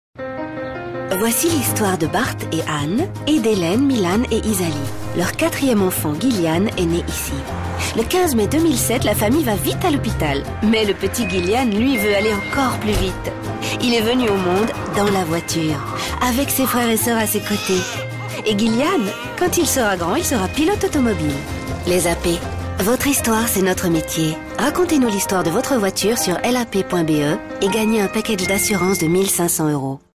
AP Assurances - matière d'assurance et de placement
Notre réseau de conseillers spécialisés est présent dans toute la Belgique. Nos clients peuvent toujours nous contacter pour tout conseil en matière d'assurance et de placement. Ou encore pour obtenir notre aide en cas de problème ou de sinistre. C'est une valeur ajoutée qu'ils apprécient.

Les AP ont déjà plus de 75 ans d'expérience dans le domaine des assurances vie, des assurances annulation, des assurances frais de funérailles, des assurances confort, des assurances soins de santé, des assurances voyages, des assurances collectives hospitalisation et beaucoup d'autres.

Si vous voulez plus de renseignements au sujet de nos assurances, n'hésitez pas de prendre contact avec les AP.